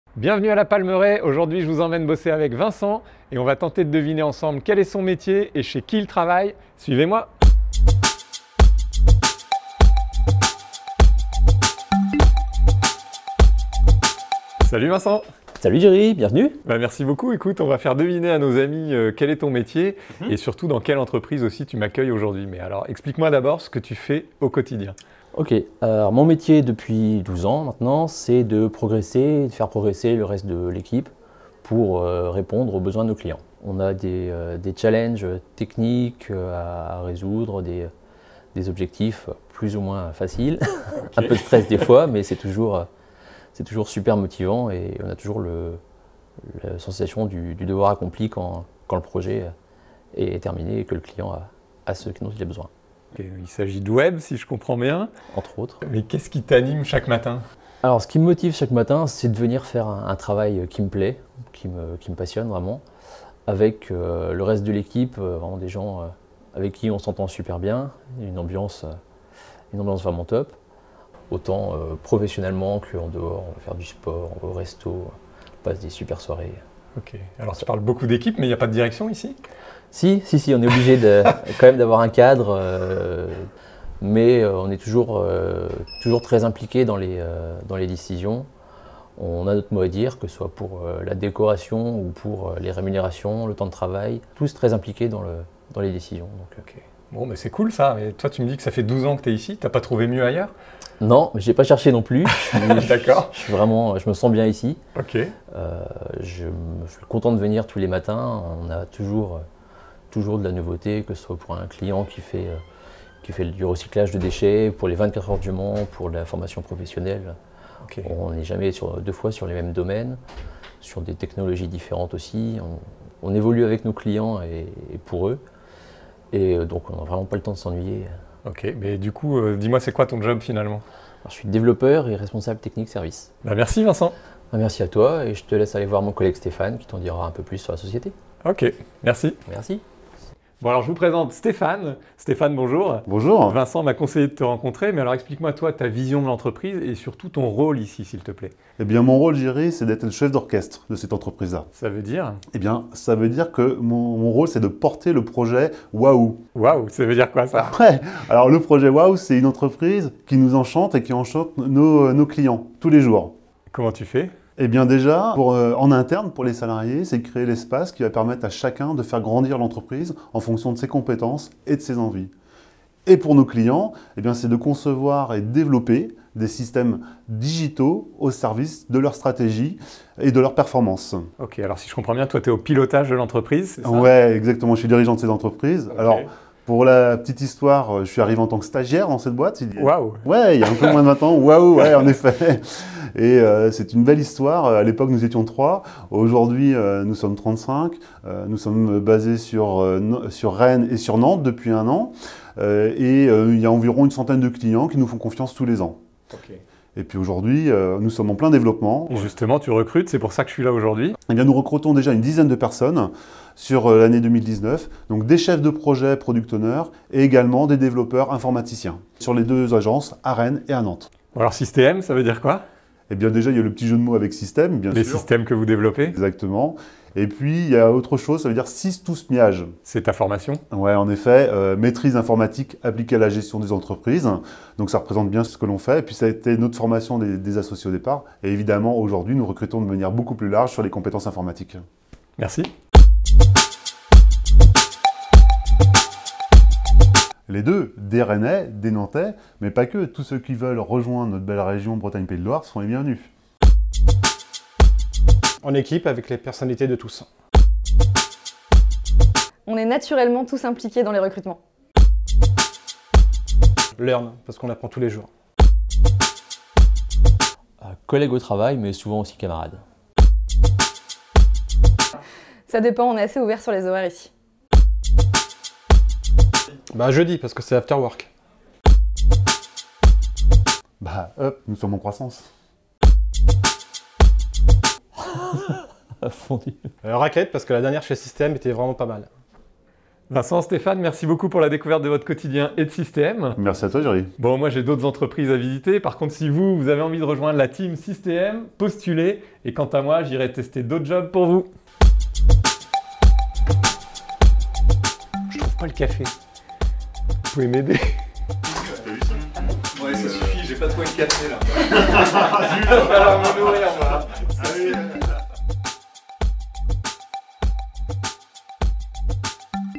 Je bosse comme développeur web avec Vincent chez 6TM
🎬Dans ce 48e épisode, je pars à la rencontre de Vincent, pour vous faire deviner son job et l'entreprise à taille humaine qui l'emploie... Bienvenue à la Palmeraie ! Son métier : "Progresser et faire progresser le reste de l'équipe pour répondre aux besoins de nos clients." Son quotidien ? "Des challenges techniques à résoudre..."
🚀Il s'agit de projets web, de conception de systèmes digitaux qui servent des stratégies d'entreprises ! Vincent travaille notamment sur le portail des 24H du Mans 👍 Il apprécie l'esprit d'équipe et l'ambiance, au travail mais aussi en dehors : sport, resto... L'entreprise s'appelle 6TM et tous les salariés prennent part aux décisions, que ce soit pour la décoration ou la rémunération, les embauches, etc.
👊S'épanouir, prendre plaisir, apprendre, co-construire 6TM, s'entraider... Voilà leur conception du travail que je partage avec vous dans cette nouvelle vidéo. Découvrez pourquoi Vincent travaille avec joie depuis 12 ans ici et pourquoi il ne veut pas en changer... Et Stéphane Trémier, chef d'orchestre de cette société, nous explique son projet "Waouh" pour enchanter le quotidien : "Créer l'espace pour permettre à chacun de faire grandir l'entreprise en fonction de ses compétences et de ses envies." Ils étaient 3 il y a 20 ans, ils sont 35 aujourd'hui sur Rennes et Nantes depuis un an.

🔎Il paraît que la filière numérique embauche... 6TM recrute ! Une dizaine de personnes. Suivez-moi dans cette nouvelle expérience de job au cœur du 6TM !